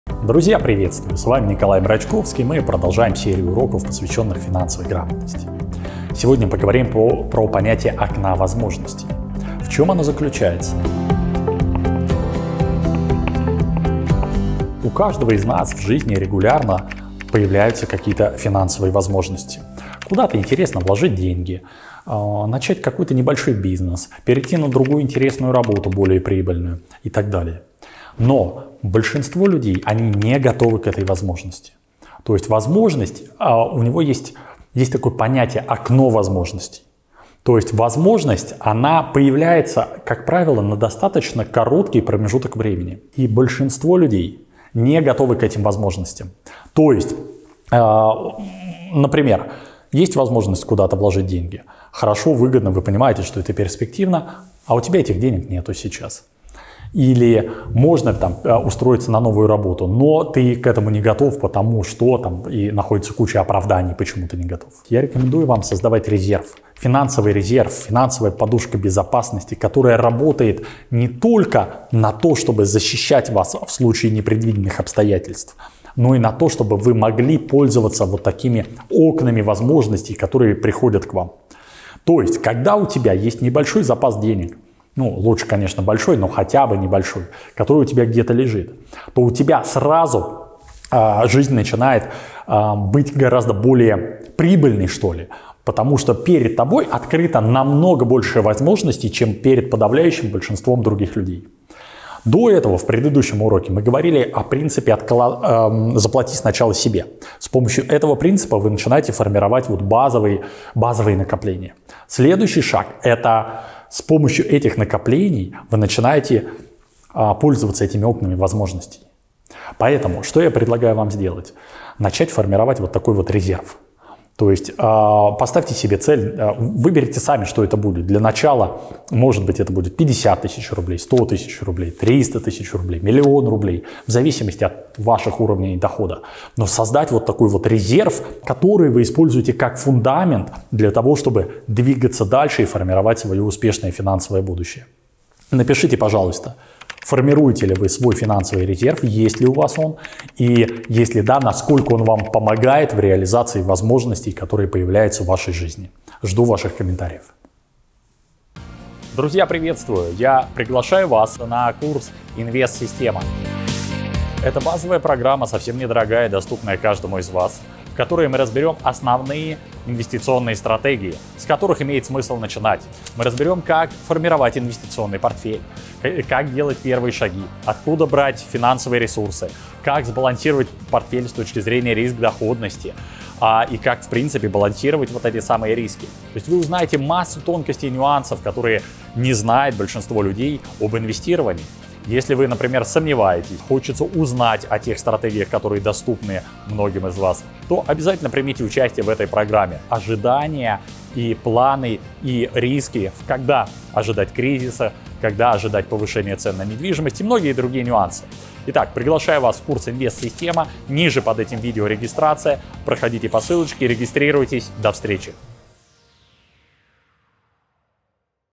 Финансовая грамотность - Урок 11: Окно возможностей. Ваш финансовый резерв (фундамент).
Финансовая грамотность - 11 Урок: 10 Урок: Окно возможностей:

Друзья, приветствую! С вами, Николай Мрочковский. Продолжаем серию уроков по финансовой грамотности. В 11 уроке мы поговорим про понятие - окно возможностей. У каждого из нас в жизни регулярно появляются финансовые возможности:
- Куда-то вложить деньги
- Начать небольщой бизнес
- Перейти на более прибыльную работу
и так далее.
НО большинство людей не готовы к этой возможности.
Окно возможностей - возникает на определенный отрезок времени, и нужно иметь ресурсы для того, чтобы успеть воспользоваться этой возможностью.

СОВЕТ: Создавайте финансовый резерв, который не только защитит вас от непредвиденных обстоятельств, но и позволит использовать ВОЗМОЖНОСТИ, что вам открываются.

Напишите пожалуйста в комментариях, формируете ли вы свой финансовый резерв, есть ли у вас он и насколько хорошо он вам помогает?
Жду ваших ответов.